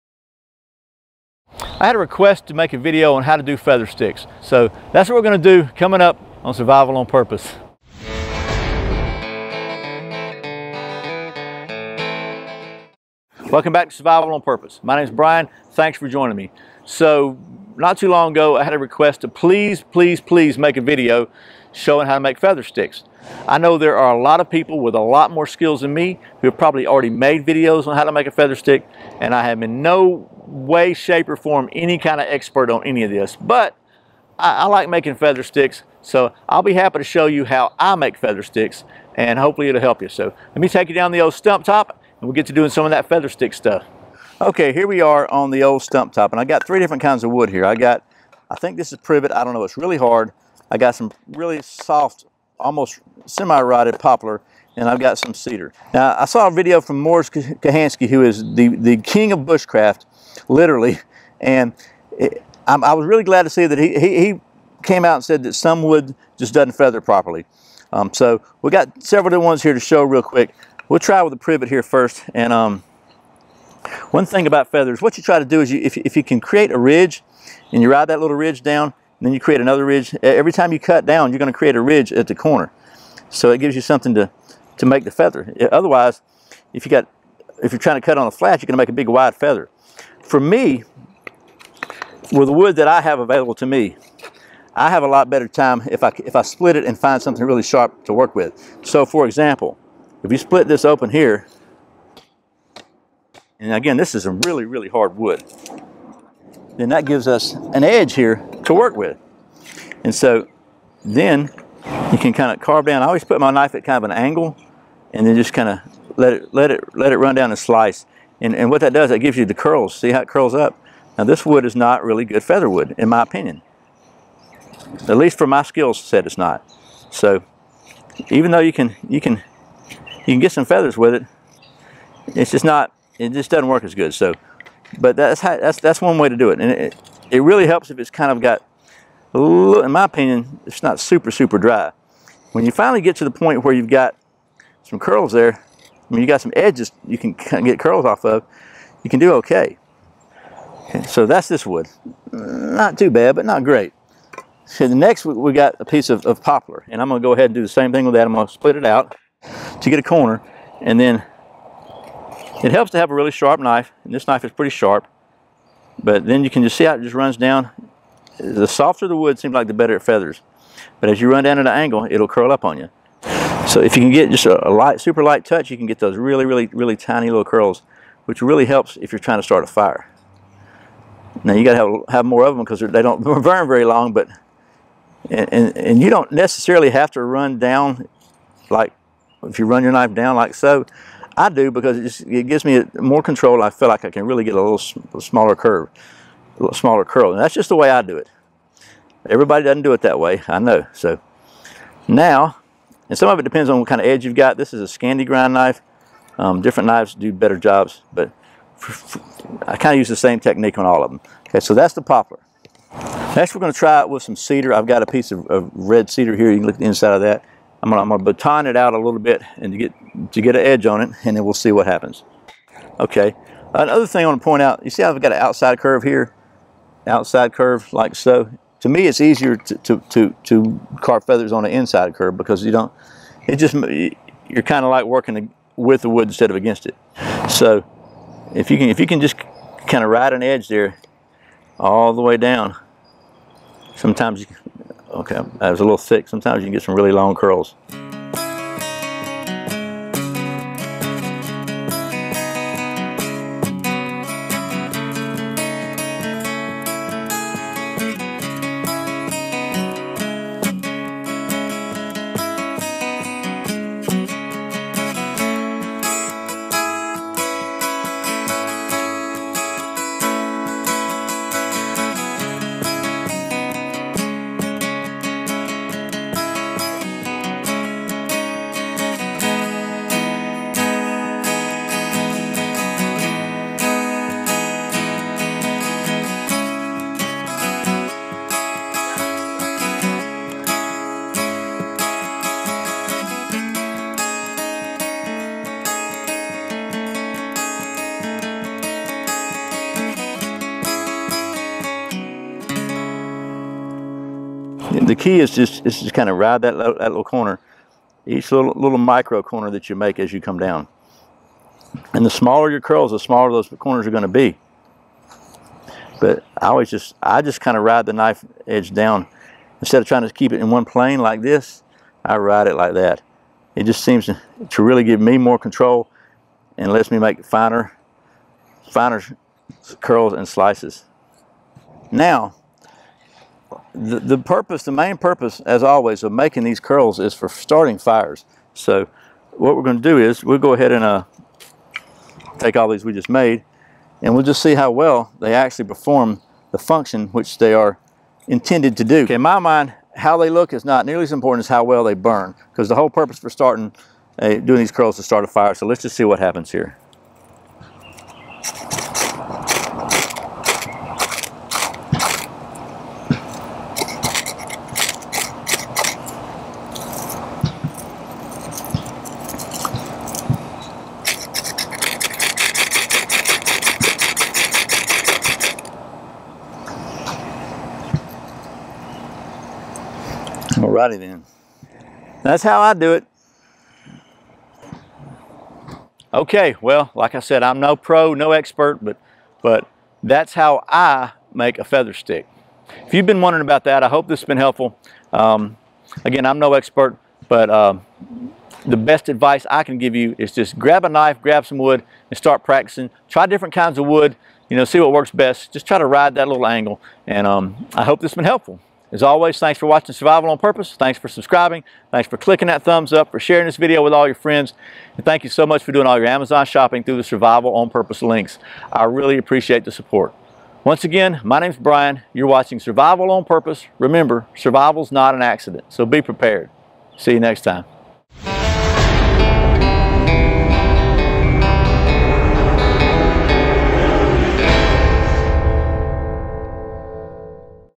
How To Make Feather Sticks - Viewer Request - Firestarter Videos
Knowing how to make a feather stick fire starter is a great bushcraft skill to know. Feather sticks make great tinder and are a good way to practice your knife skills.

Please consider supporting this channel by using this link for all your Amazon shopping